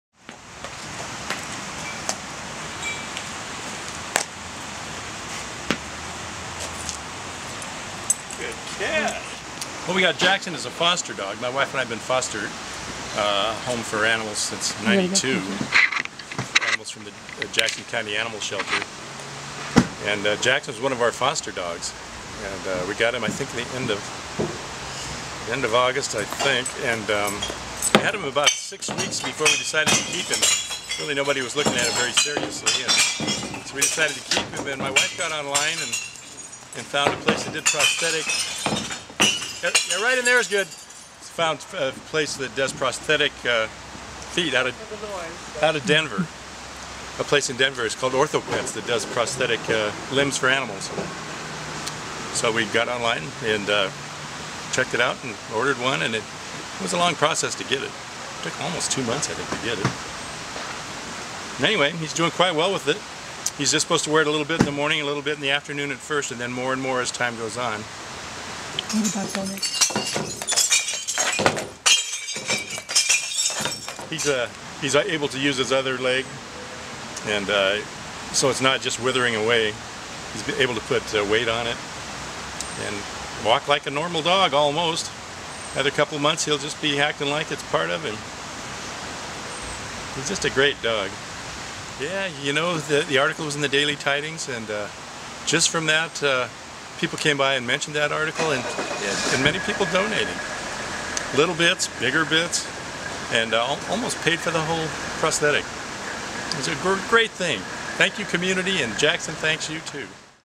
jaxon
Ashland, Ore., -- Christmas came early for Jaxon, a rescued three-legged canine thats been waiting on a prosthetic limb since late September.  Tim and Lucinda Church rescued the 1-year-old black labrador-great Dane mix from the Jackson County Animal Care & Control Center in August. Since 1992, Tim Church has helped find homes for more than 200 animals by fostering them at the center.

Just after Thanksgiving, Jaxons prosthetic leg arrived and he has been running on it since. Bounding across the parking, Jaxon easily fields one of a dozens well-worn tennis balls to be found around the center.

Church said the quality of Jaxons life has improved since getting the prosthetic leg.